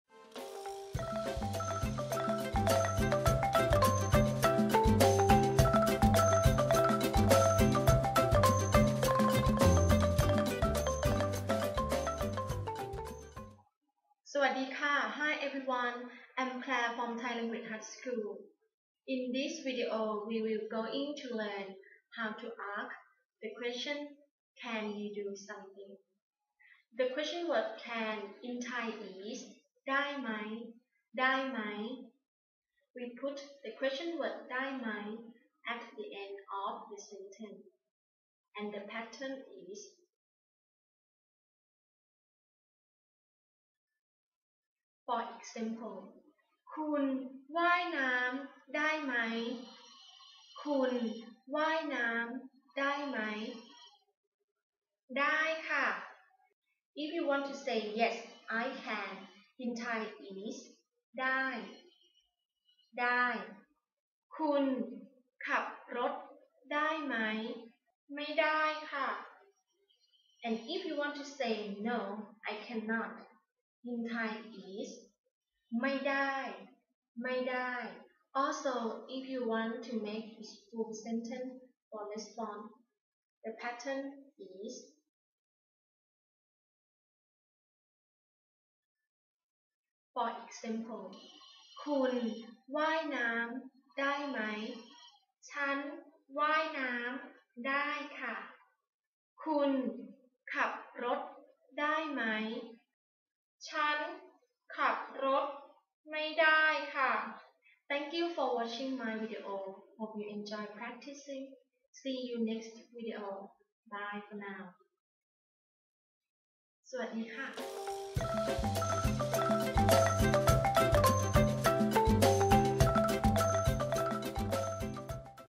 Learn Thai Question Word | Can you do something? | ได้ไหม
In this Learn Thai online lesson we will talk about the Thai Question word ได้ไหม Can you do some thing?. I hope you enjoy our Learn Thai online lesson.

We do lessons at our school, at a site of your choice in Bangkok or learn Thai Online using Skype.